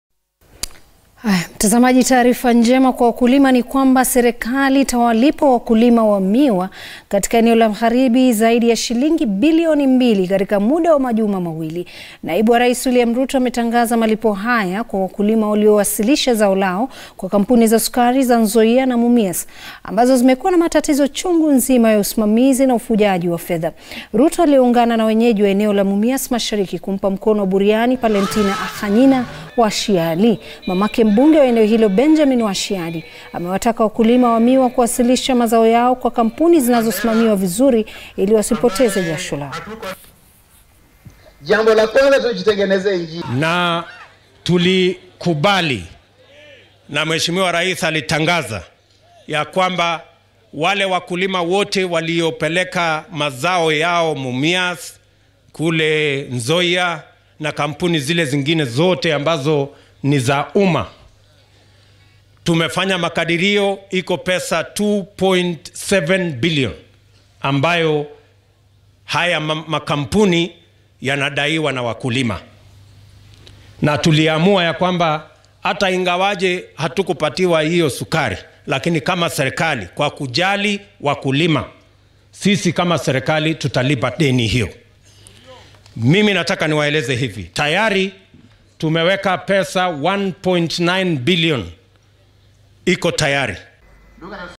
Wakulima wa miwa kulipwa
Serikali itawalipa wakulima wa miwa katika eneo la magharibi zaidi ya sh. 2b katika muda wa majuma mawili. Naibu wa rais william ruto ametangaza malipo haya, kwa wakulima waliowasilisha zao lao kwa kampuni za sukari za Nzoia na Mumias,